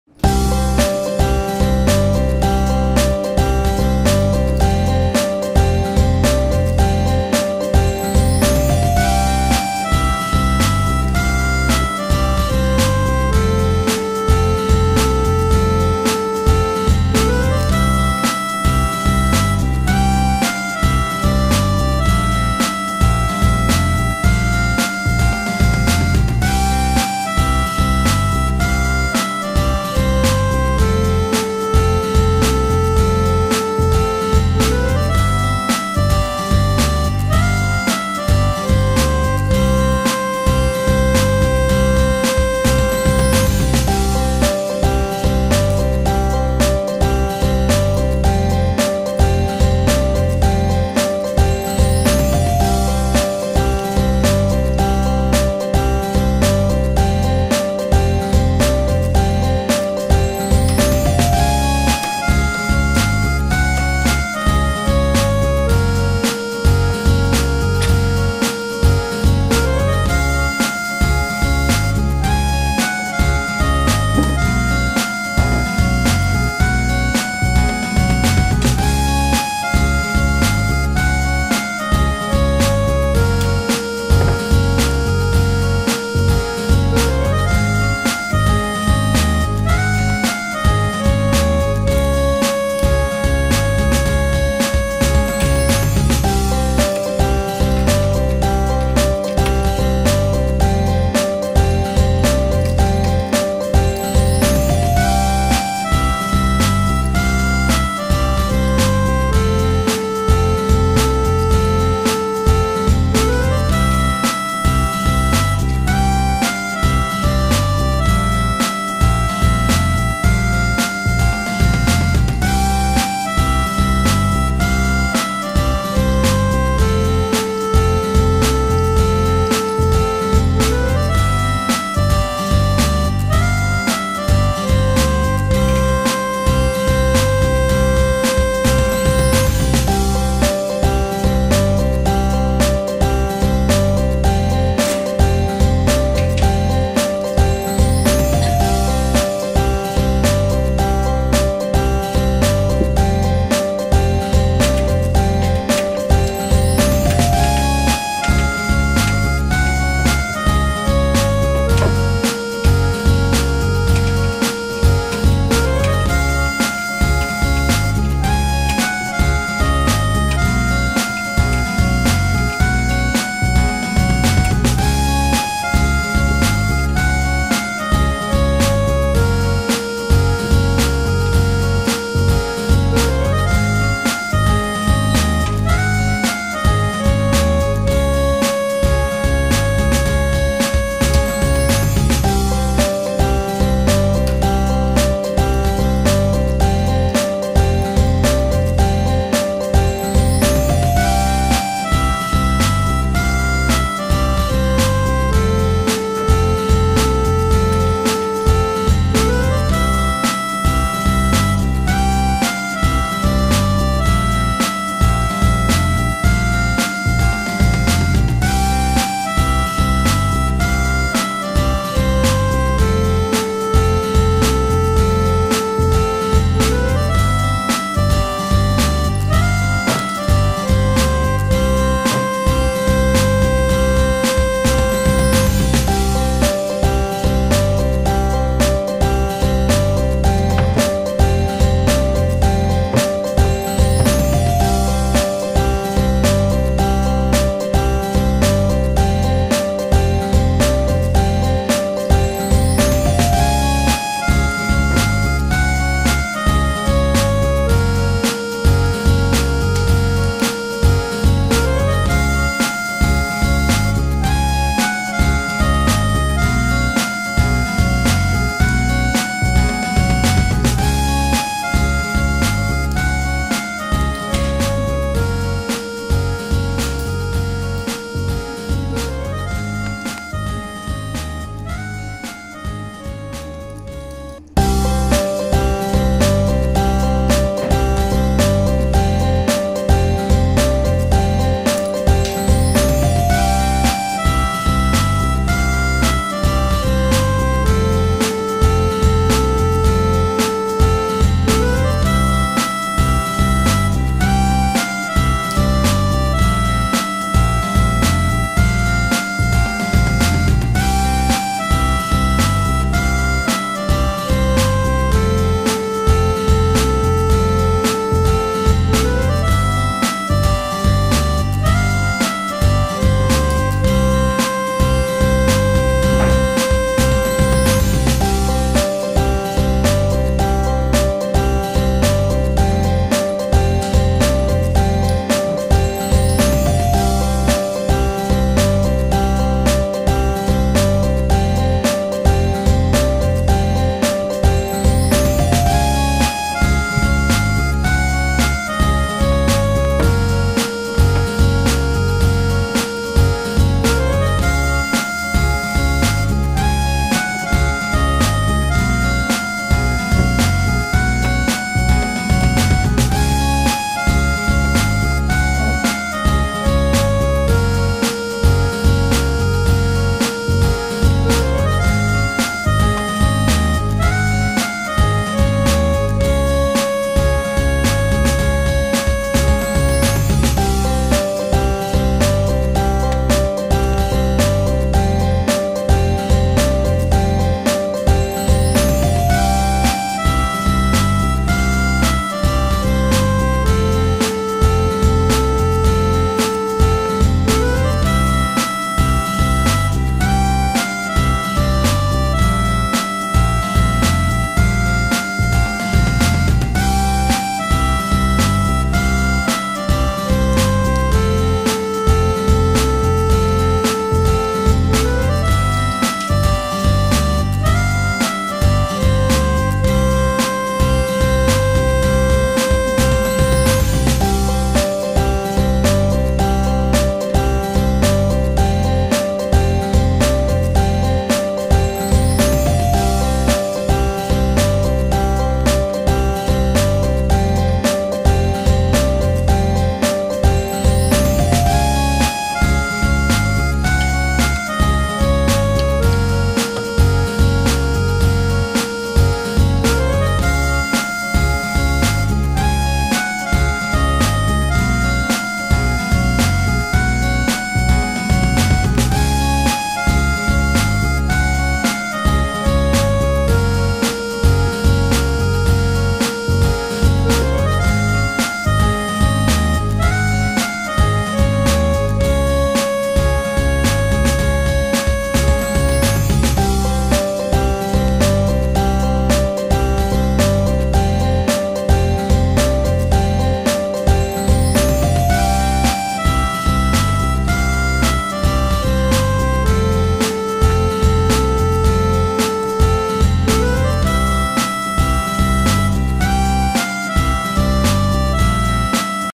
Beben, lustige Tage mit Kätzchen und Enkelkatzen - #0196
Lustige Tage mit Beben, Kätzchen und Enkelkatzen

Werden Sie Mitglied und unterstützen Sie das Katzenteam „Beben“, unterhalten Sie Beben und seine Kinder, kaufen Sie Leckereien und mehr. Vielen Dank.